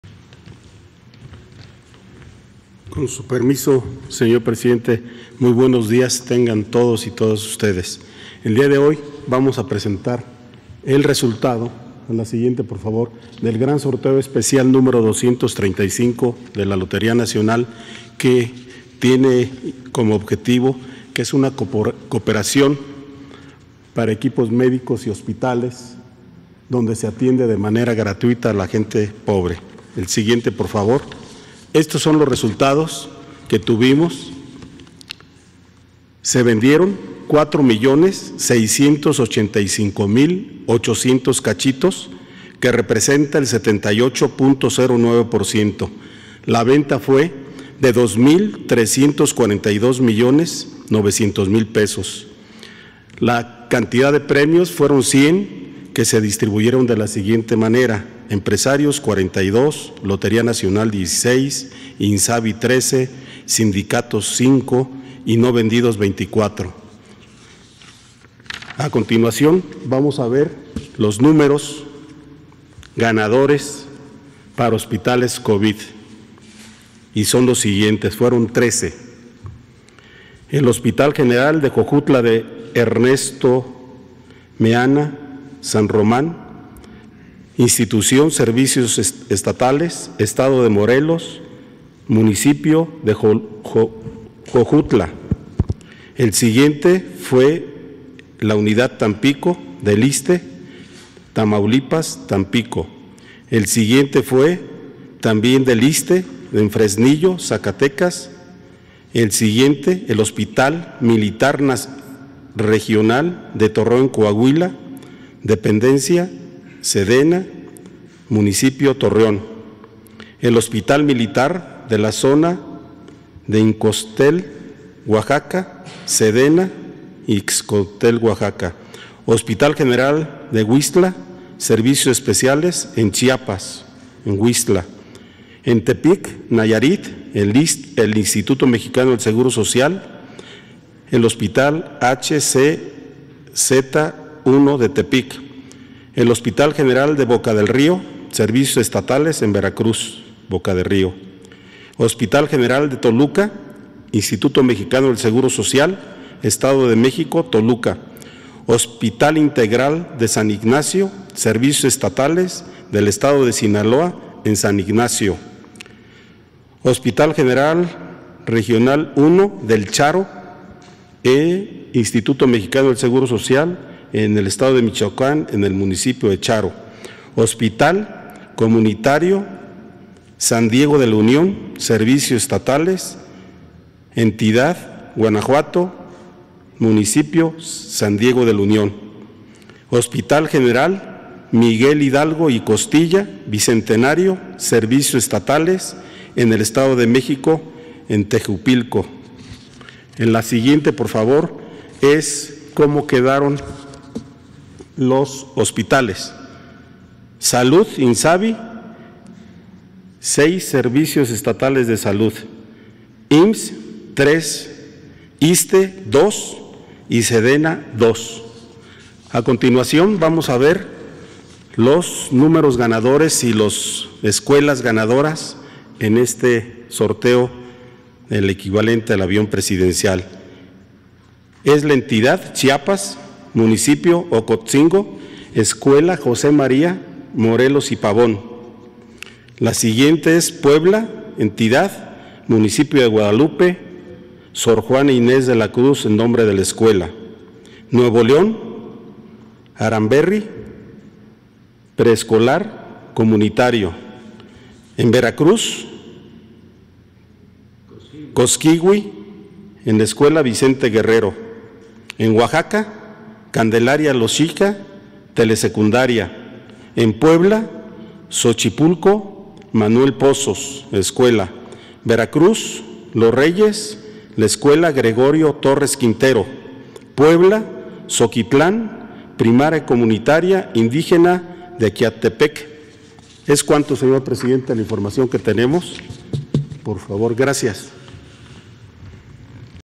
SE VENDIERON EL 78.09% DE CACHITOS DE LA RIFA DEL AVIÓN
El director de la Lotería Nacional Ernesto Prieto dio a conocer los pormenores de la rifa del avión presidencial. Reveló que se vendieron 4,685,800 cachitos, el 78.09% del total y que se recaudó 2,342,900,000